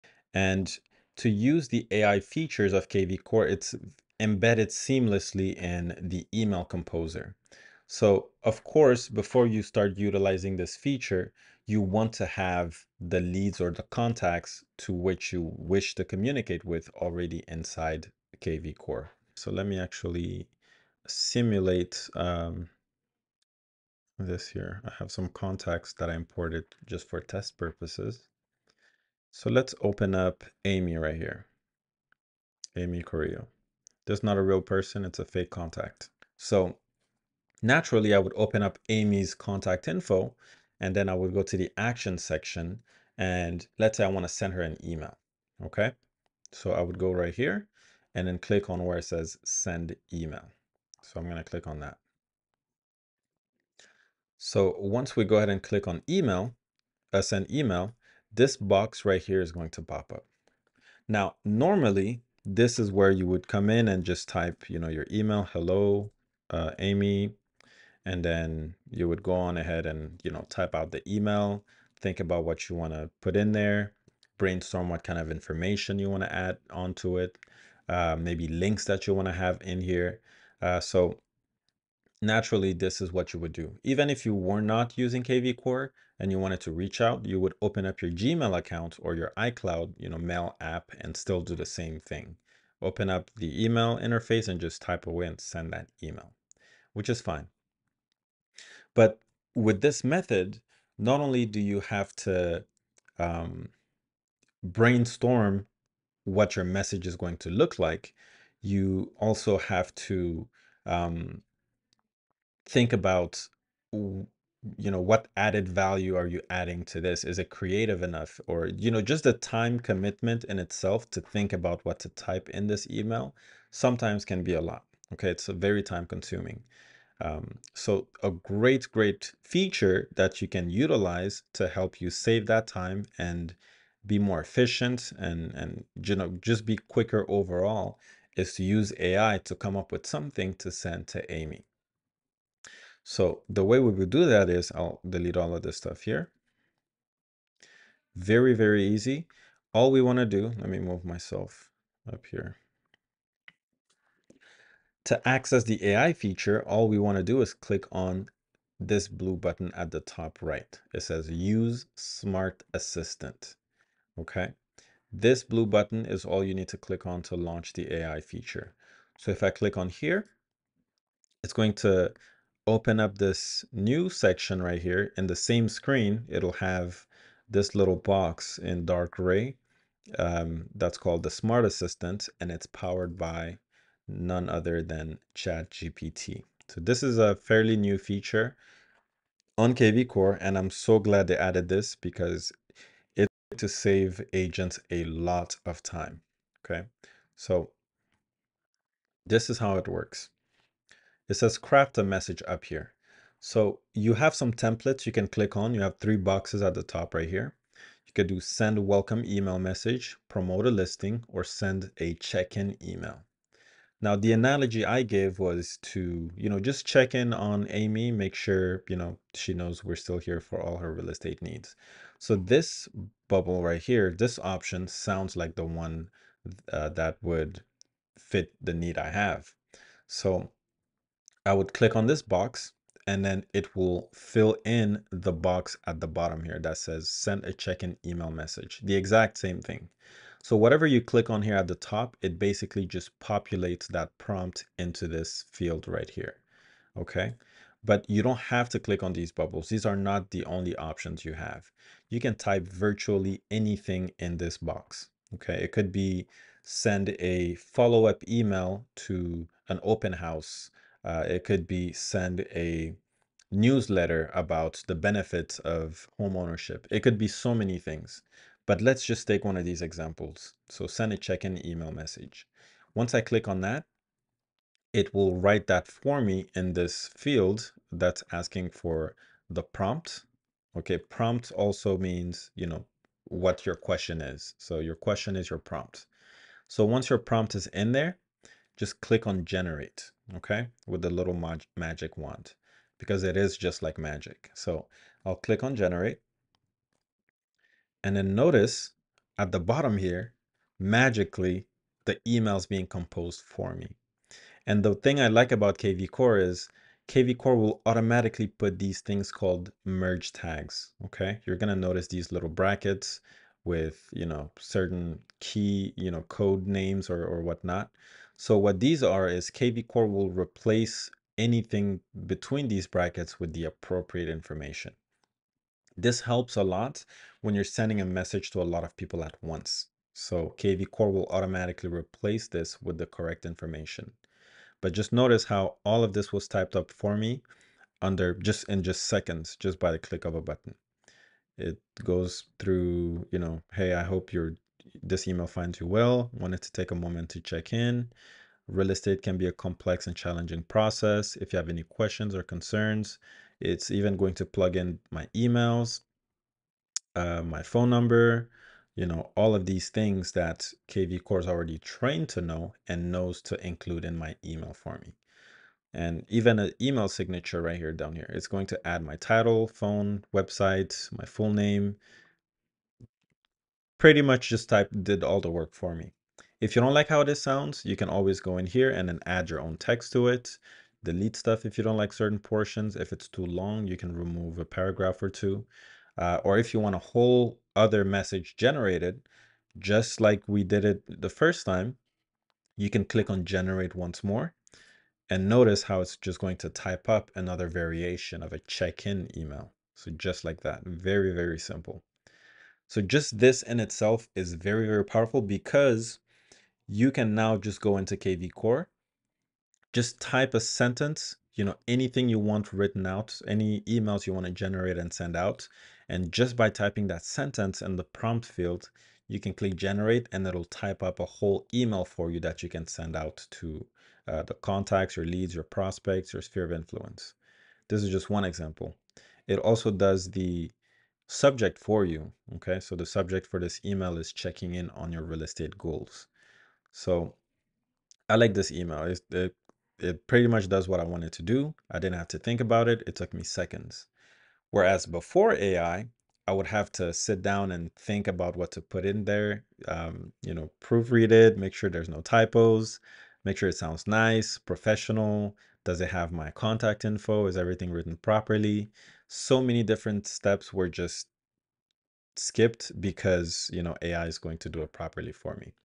Create EFFECTIVE Marketing Campaigns Using KvCORE AI
Unlock the power of AI in your marketing strategy! 🚀
Join us in this video as we explore how to generate highly effective marketing campaigns using KvCORE AI. Discover the seamless integration of AI features, including smart assistants and personalized content creation, to elevate your marketing efforts. Learn valuable tips and tricks to make the most of KvCORE's AI capabilities and take your campaigns to new heights. Don't miss out on this game-changing insight into leveraging AI for marketing success!

Become A member of My Group to access our weekly Zoom Training, live in-person events, and more! 👇🏾